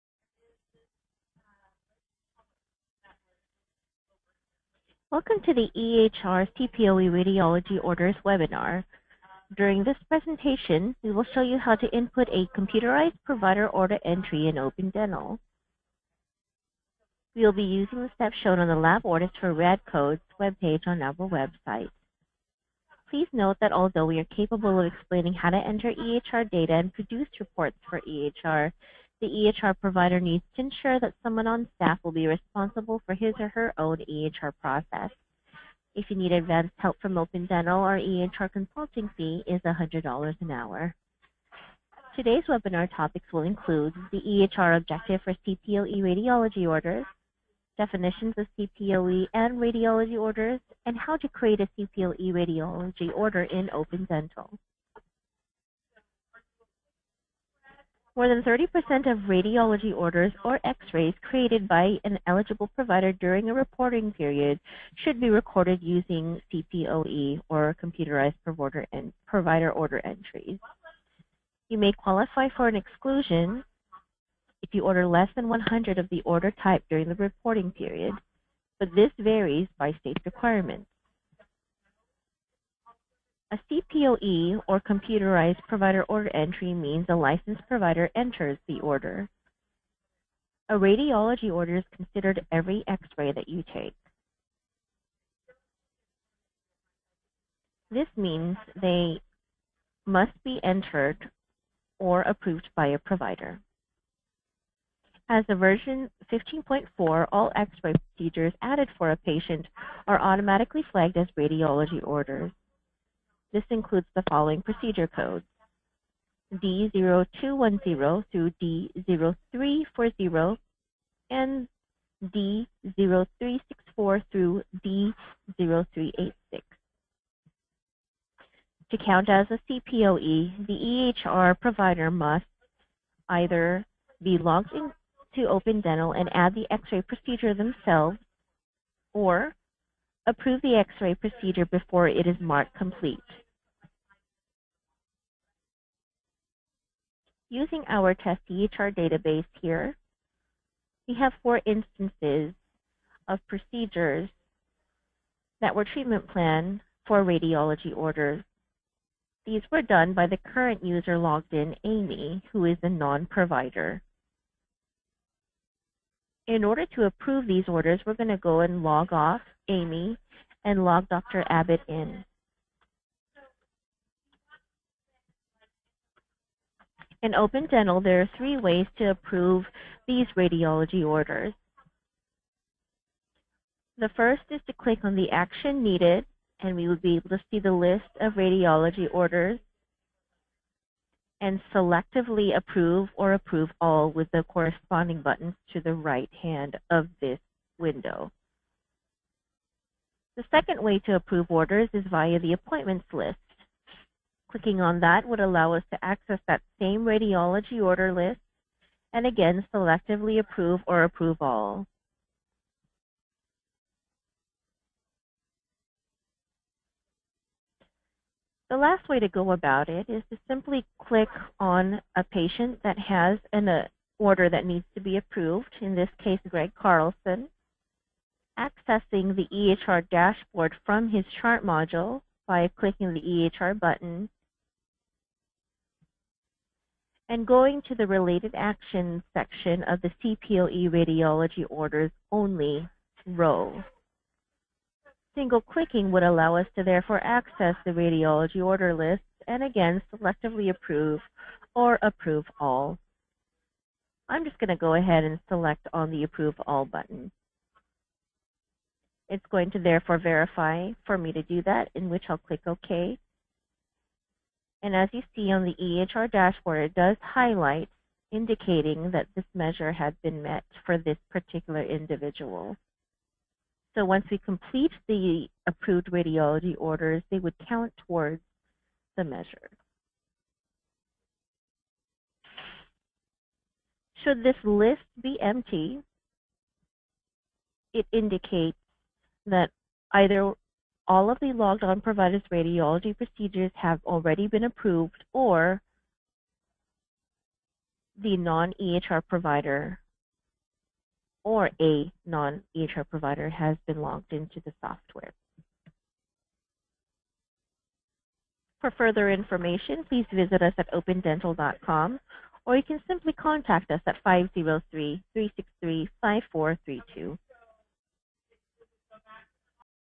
Open Dental Webinar- EHR: CPOE Radiology Orders
Learn how to input a Computerized-Provider Order Entry (CPOE) Radiology Order in Open Dental.

Topics will include:
- EHR Objective for CPOE Radiology Orders
- Definitions of CPOE and Radiology Orders
- Create a CPOE Radiology Order